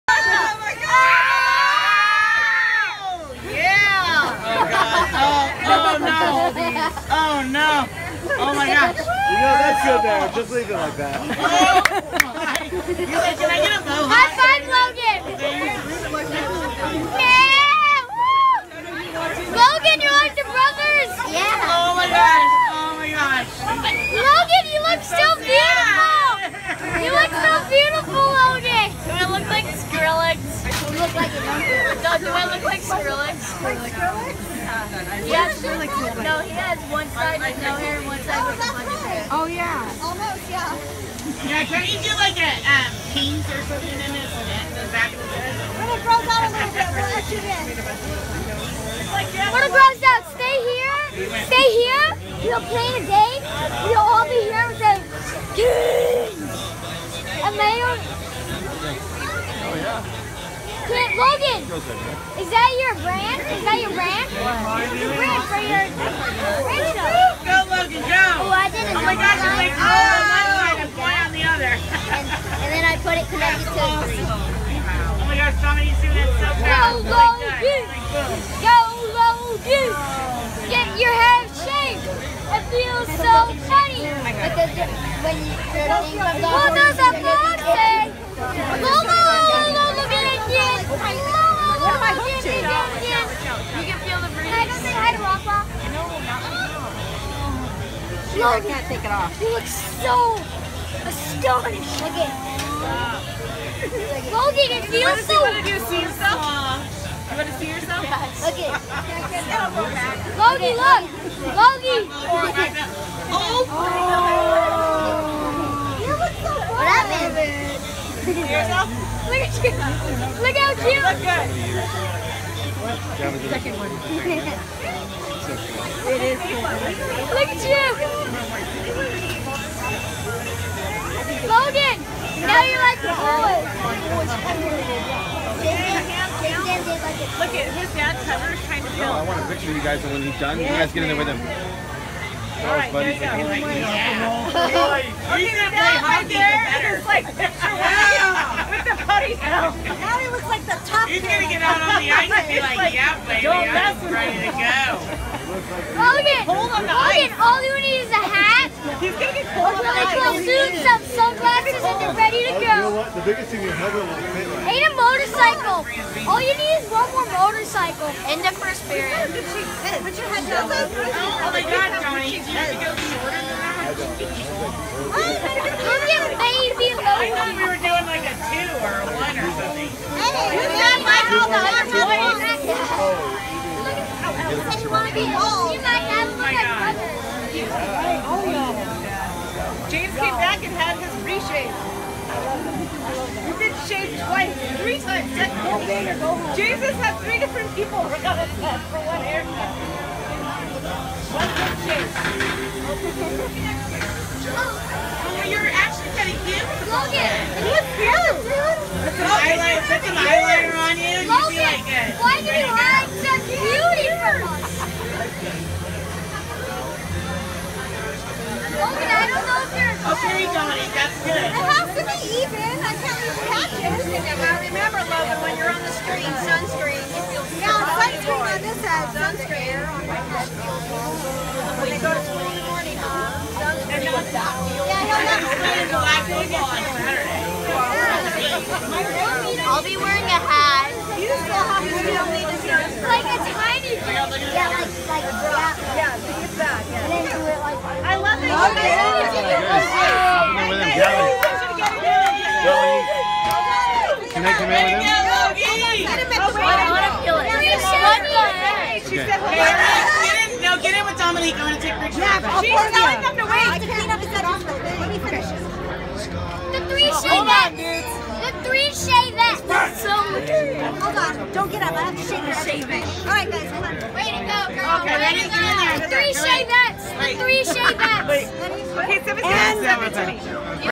Logan Fessenden's Haircut - St Baldrick's, Old Town Monrovia, CA 03/17/16
Logan Fessenden "wanted to help out", so along with 14 friends from Team Mayflower School got his haircut at BUZZFest in Old Town Monrovia, CA for St Baldrick's Foundation a full 501(c)3.

St Baldrick's raises money through donations for pediatric cancer research as well as make wigs with the donated hair.